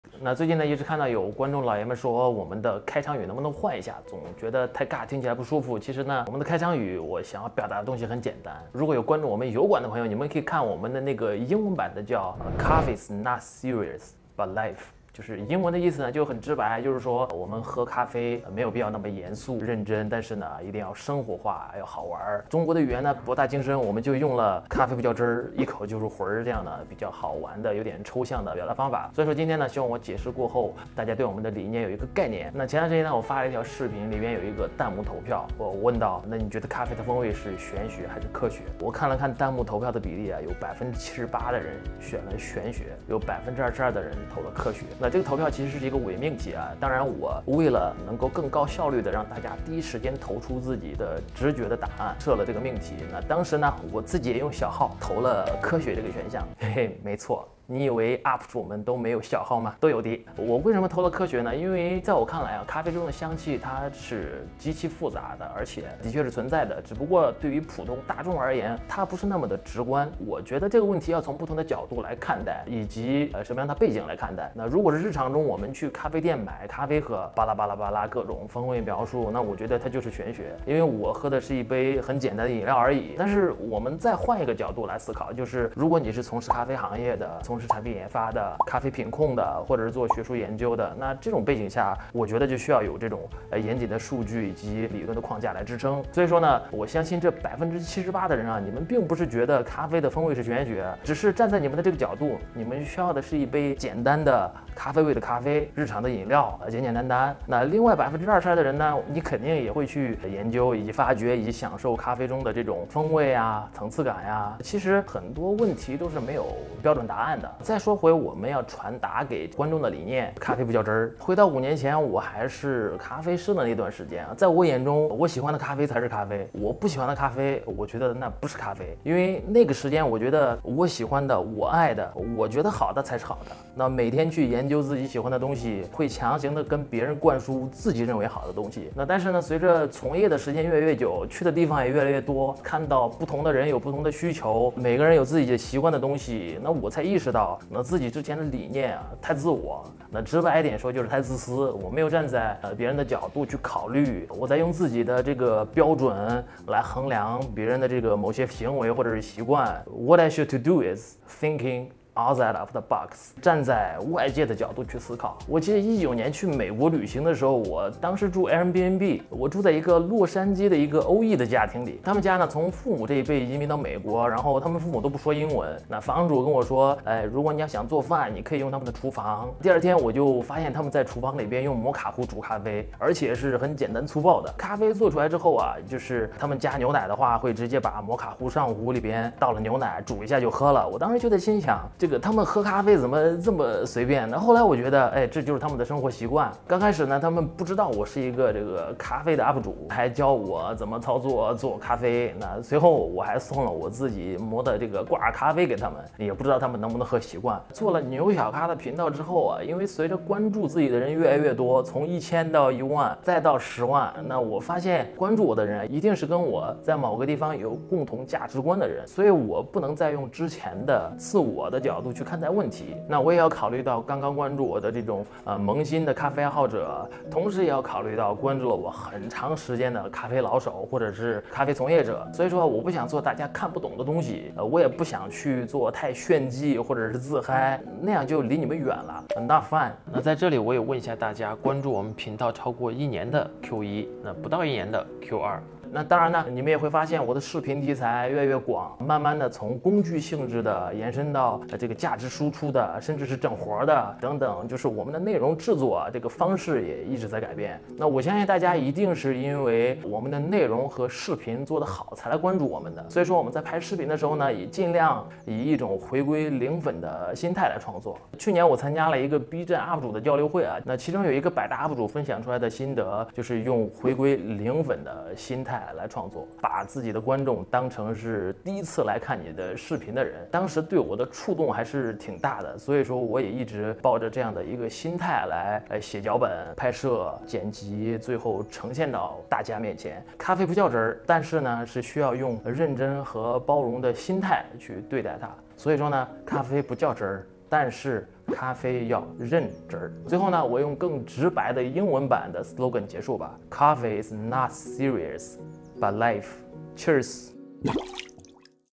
進來聊聊！你覺得咖啡風味是玄學 or 科學？| 牛小咖MumaMoo
你覺得咖啡風味是玄學 or 科學？
頻道首頁有更多精彩內容哦！